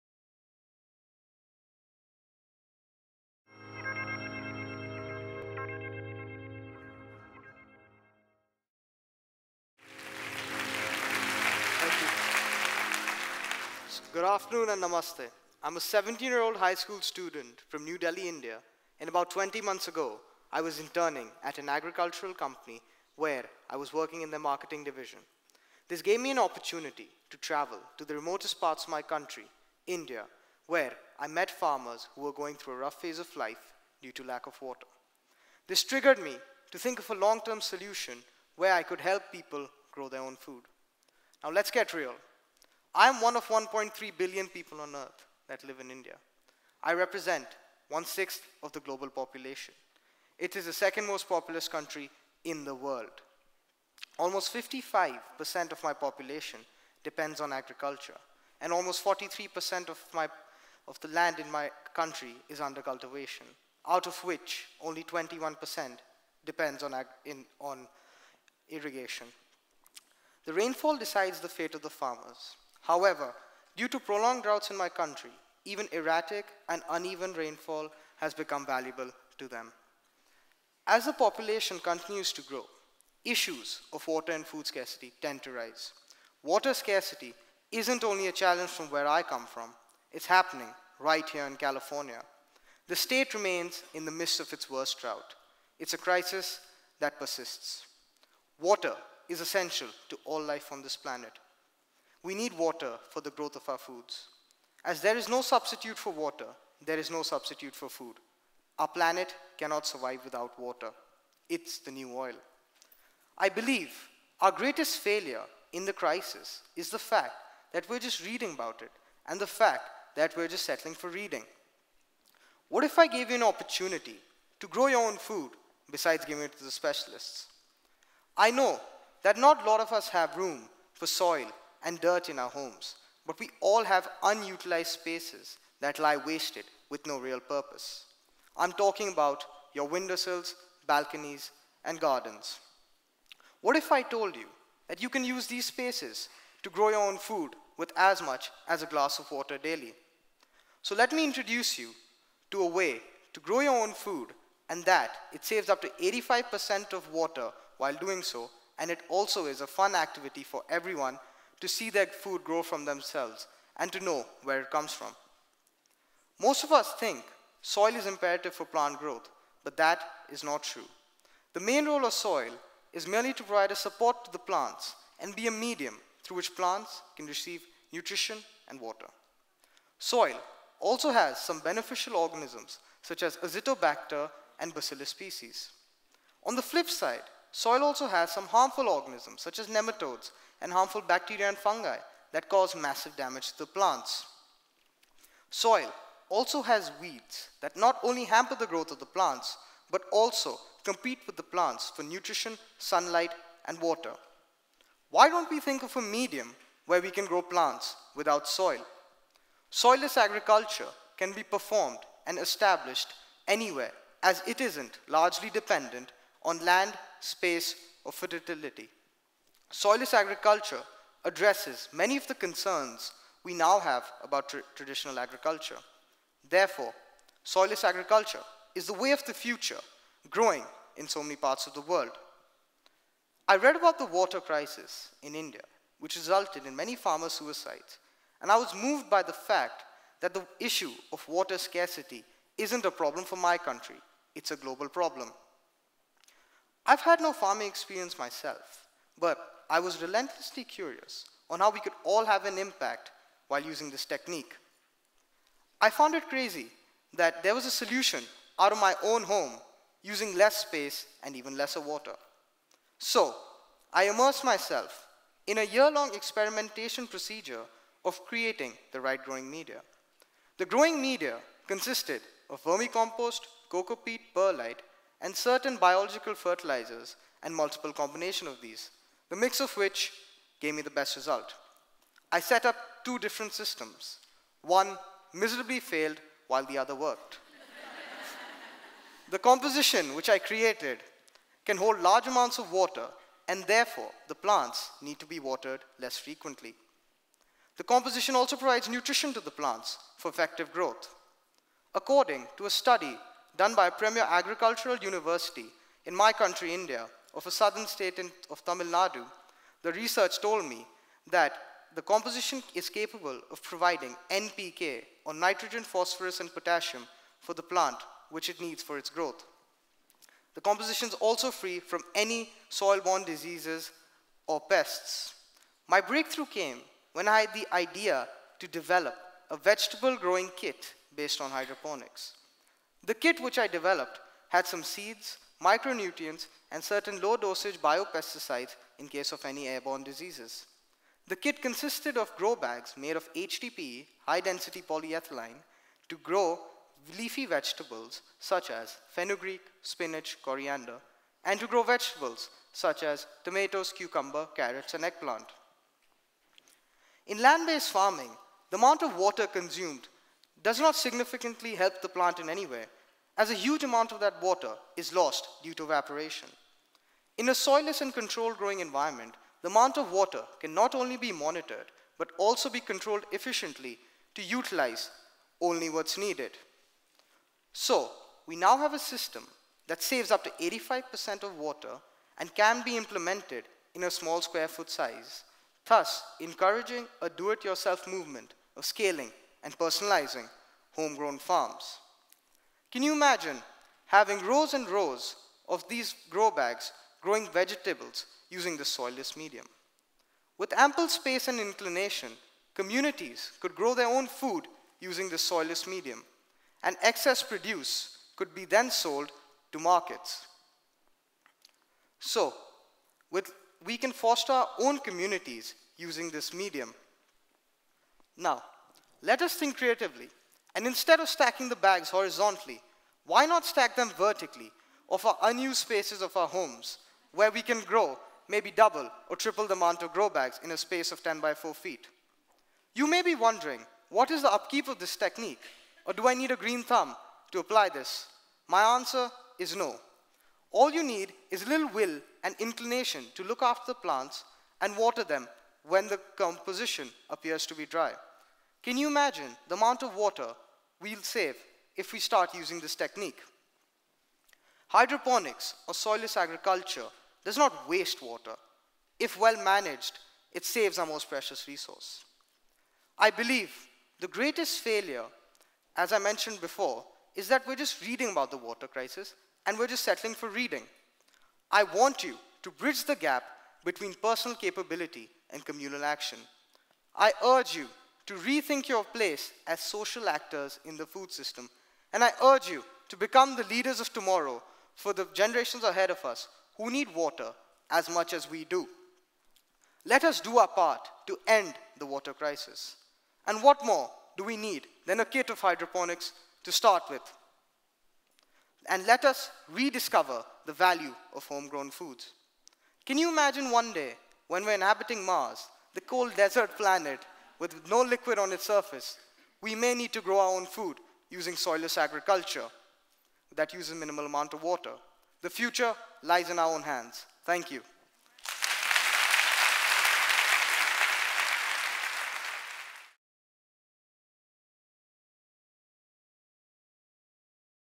Troubled Water: Time to Conserve Our Resources | Shantanu Singhal | TEDxPasadenaWomen
Shantanu Singhal, a teenage boy from New Delhi, reveals a revolutionary plan for growing food with very little water and absolutely no soil. His straightforward invention of "soilless agriculture" might not only change how the world deals with drought, but how we might someday grow produce on Mars.

When the Nepal earthquake struck in April 2015, the destruction was so absolute that it was impossible for Shantanu to remain a passive witness. With the support of family and a knack for marketing, he began a social media campaign to request help. His team raised 16,000,000 Indian Rupees (approx. $25,000) plus material donations, and he personally visited the worst affected areas to hand over the proceeds. His essay on the experience was published in national newspaper The Indian Express.
After reading about rising farmer suicides in India due to drought, Shantanu created UNNAT, a hydroponics project and experiment in soilless agriculture. One key outcome was the development of a simple kit which can be used to grow several essential crops such as fenugreek, cabbage and spinach. The state government of Uttar Pradesh noticed the potential of UNNAT, and selected the organization for a pilot study that began in August 2016.
Shantanu also organizes school leadership events and talks, se